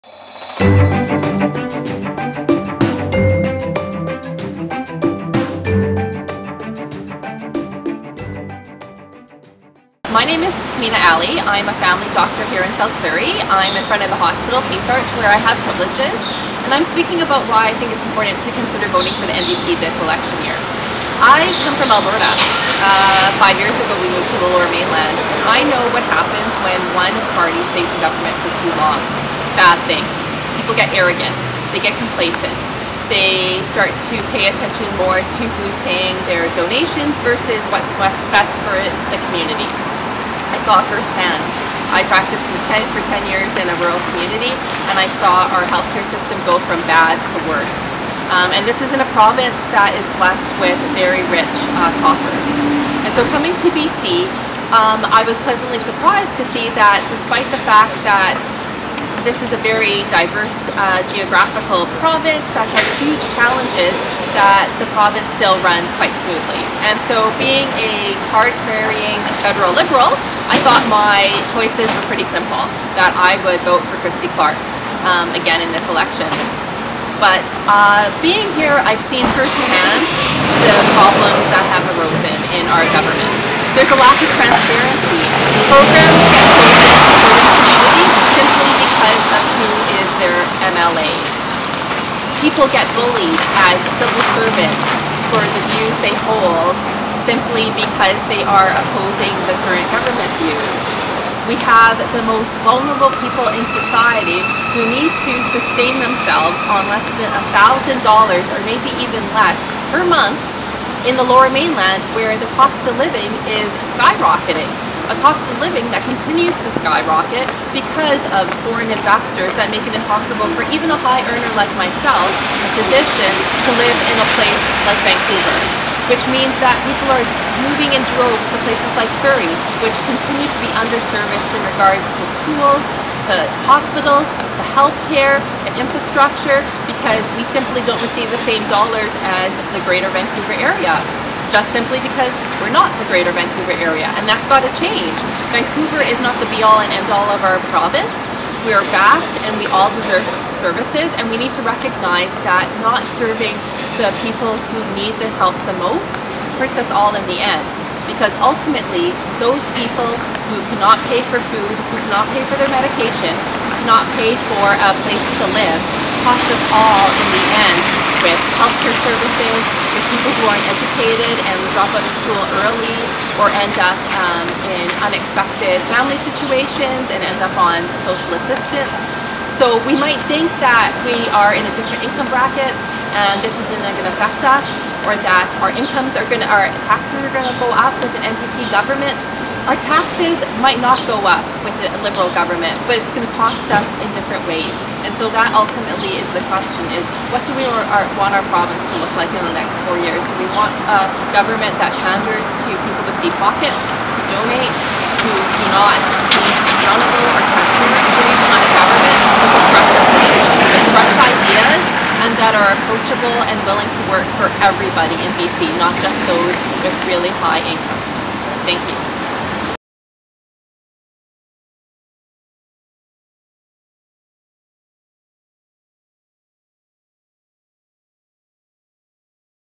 DR Tahmeena
DR Tahmeena is speaking about why she support the NDP